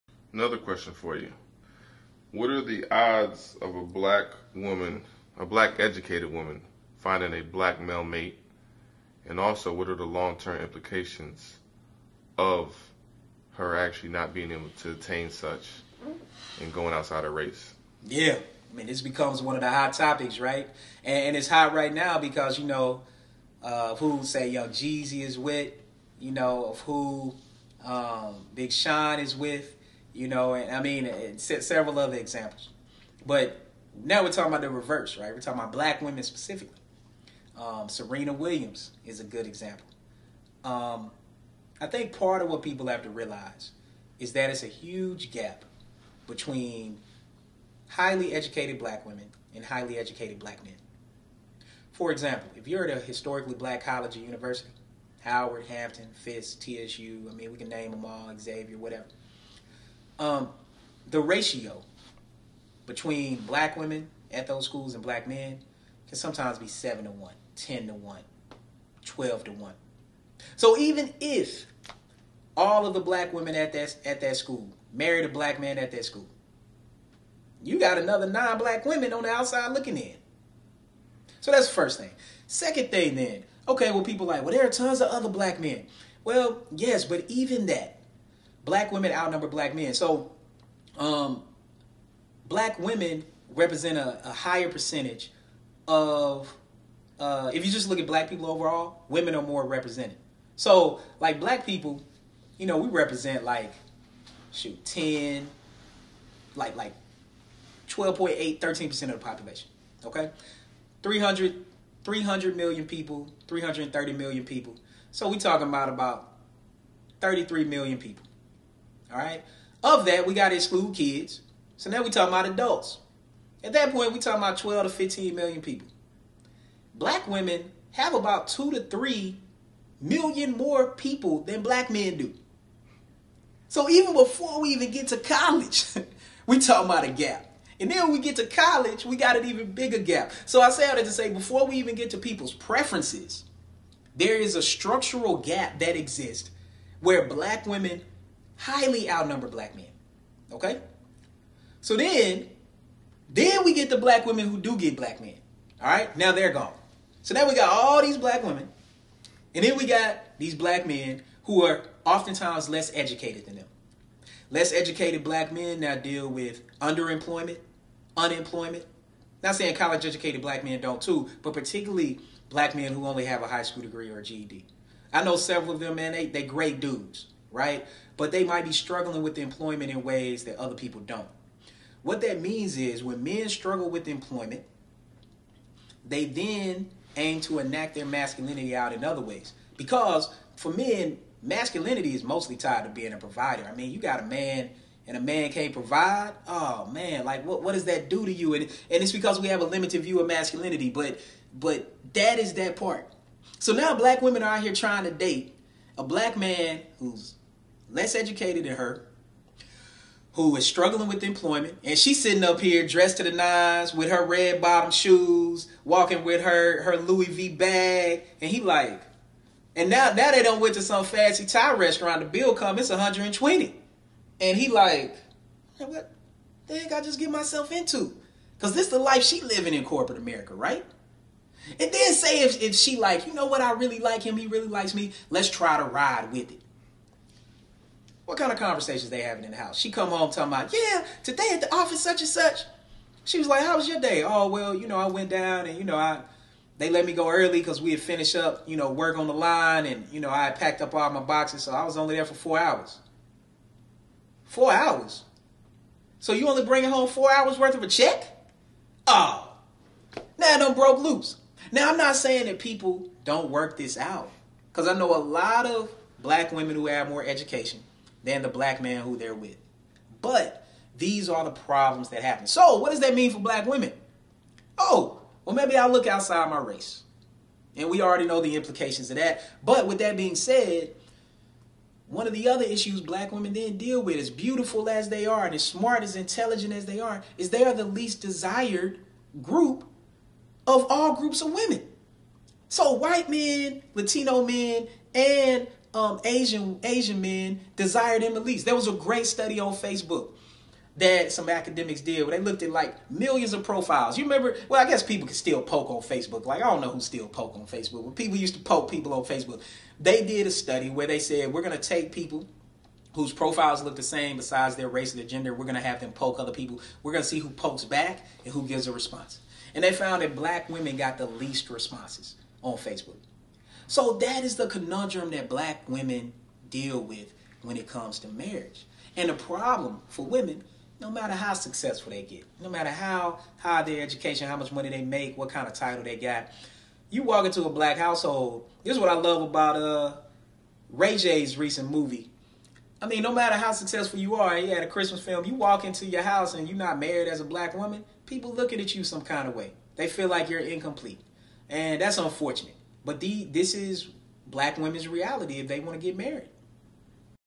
WILL EDUCATED BLACK WOMEN MARRY A BLACK MAN?
Adding some stats to the relationship debate. Purchase one of my recent books, “How Families Matter: The Simply Complicated Intersections of Race, Gender, and Work.”